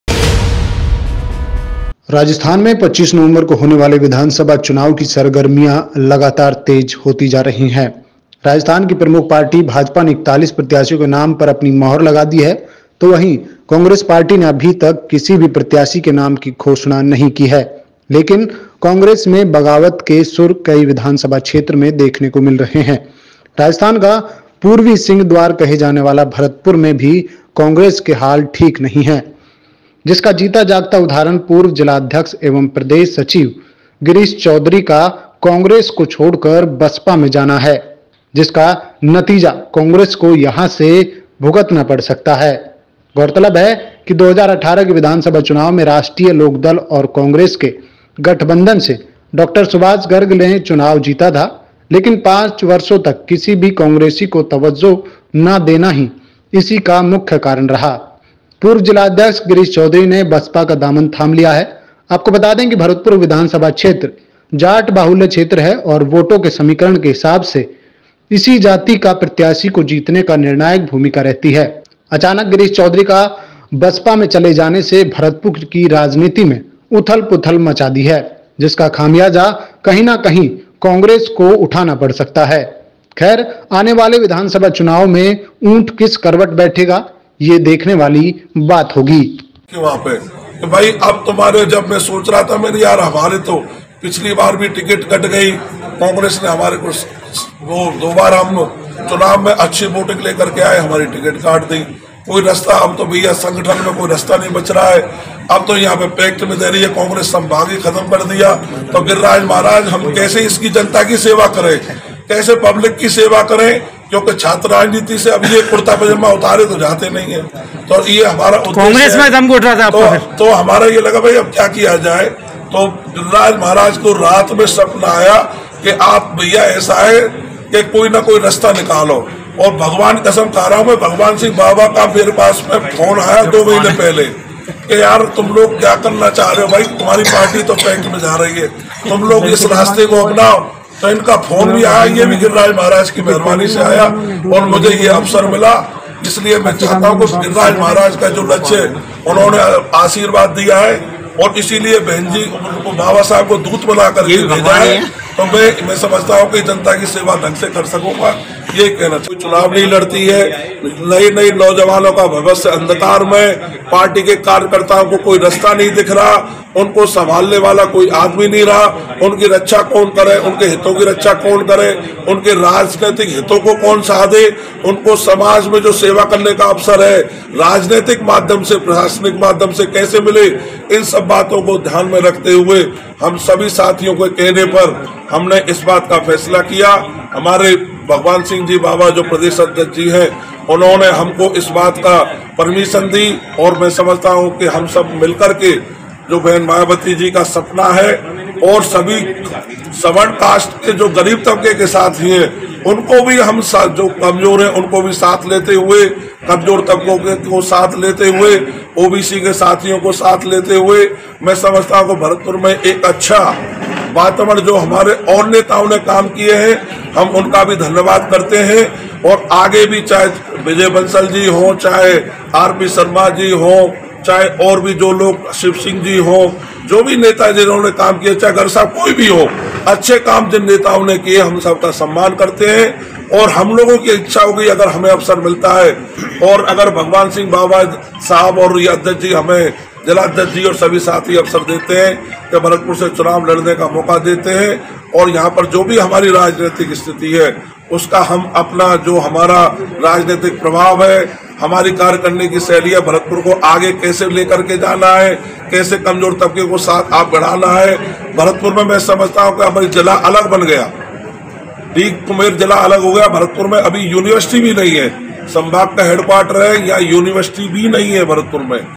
भरतपुर से Congress का बड़ा विकेट गिरा , दिग्गज नेता BSP में शामिल
विधानसभा चुनावों की  सरगर्मियां लगातार तेज होती जा रही हैं

कांग्रेस के प्रदेश सचिव गिरीश चौधरी कांग्रेस छोड़ बसपा में शामिल

पांच वर्षों तक किसी भी कोंग्रेसी को तवज्जो नहीं मिली | गिरीश चौधरी

अचानक गिरीश चौधरी का बसपा में जाना राजनीति में बड़ी उथल पुथल है

राजस्थान में कांग्रेस को बड़ा झटका, दिग्गज नेता BSP में शामिल, भगवान सिंह बाबा ने किया वेलकम

भरतपुर मनीष शर्मा की रिपोर्ट

राजस्थान में 25 नवंबर को होने वाले विधानसभा चुनावों की  सरगर्मियां लगातार तेज होती जा रही हैं। राजस्थान की प्रमुख पार्टी भाजपा ने 41 प्रत्याशियों के नाम पर अपनी मोहर लगा दी है तो वहीं कांग्रेस पार्टी ने अभी तक किसी भी प्रत्याशी के नाम की घोषणा नहीं की है, लेकिन कांग्रेस में बगावत के सुर कई विधानसभा क्षेत्रों में देखने को मिल रहा है।

राजस्थान का पूर्वी सिंह द्वार कहे जाने वाला भरतपुर में भी कांग्रेस के हाल ठीक नहीं दिखाई दे रहे हैं। जिसका जीता जागता उदाहरण पूर्व जिलाध्यक्ष एवं प्रदेश सचिव गिरीश चौधरी का कांग्रेस को छोड़कर बसपा में जाना है। जिसका नतीजा कांग्रेस को यहाँ से उठाना पड़ सकता है।

 गौरतलब है कि 2018 के विधानसभा चुनावों में राष्ट्रीय लोकदल और कांग्रेस के गठबंधन से डॉक्टर सुभाष गर्ग ने चुनाव जीता था लेकिन पांच वर्षों तक किसी भी कांग्रेसियों को तवज्जो ना देना ही इसी का मुख्य कारण है कि पूर्व जिलाध्यक्ष गिरीश चौधरी ने बसपा का दामन थाम लिया है।

आपको बता दें कि भरतपुर विधानसभा क्षेत्र जाट बाहुल्य क्षेत्र है और वोटों के समीकरण के हिसाब से भी इसी जाति का प्रत्याशी को जिताने में निर्णायक भूमिका रहती है। अचानक गिरीश चौधरी का बसपा में चले जाने से भरतपुर की राजनीति में उथल पुथल मचा दी है।

जिसका खामियाजा कहीं न कहीं कांग्रेस को उठाना पड़ सकता है। खैर आने वाले विधानसभा चुनाव में ऊँट किस करवट बैठेगा ये देखने वाली बात होगी।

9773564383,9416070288